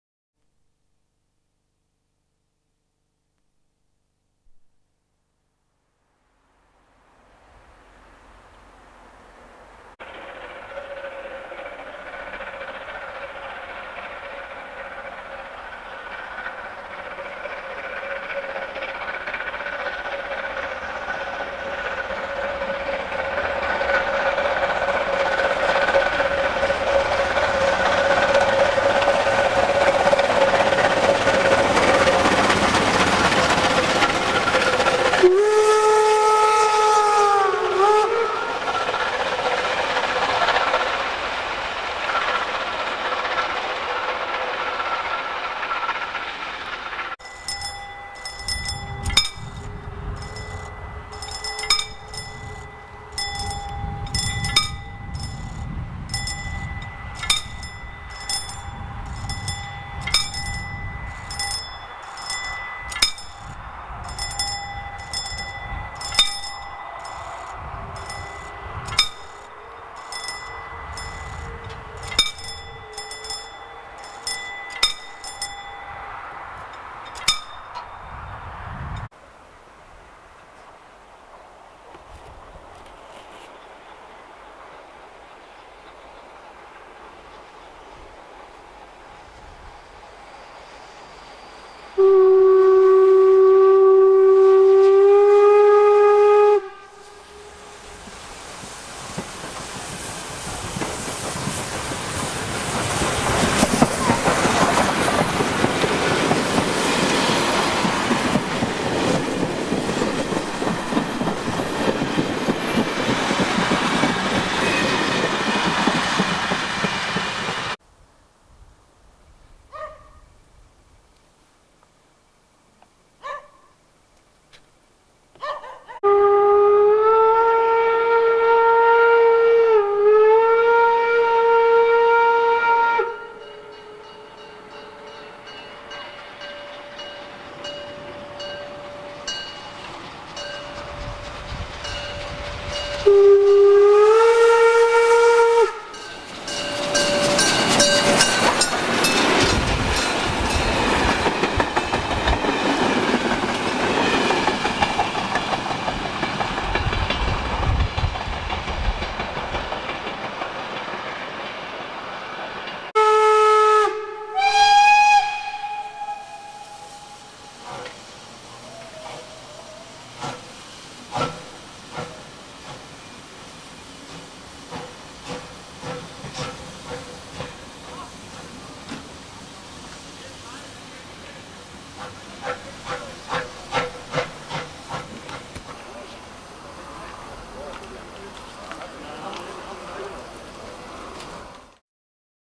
Aus meinem Bahnfilmarchiv - Teil 1 - 52 8141-5
Am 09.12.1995 unternahmen die Ostsächsischen Eisenbahnfreunde eine Rundfahrt von Löbau (Sachsen) über Ebersbach - Neugersdorf bis Zittau und zurück über Herrnhut nach Löbau. Ich hatte diesen Zug zusammen mit meinem Kumpel (Fahrer) per Auto verfolgt und konnte so Aufnahmen in Niederkunnersdorf (BÜ) und am Haltepunkt  Großschweidnitz machen. Anschließend waren wir noch nach Löbau ins BW gefahren, wo sich die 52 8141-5 ein Stelldichein mit einer BR 95 gab, die dort gerade zu Gast war.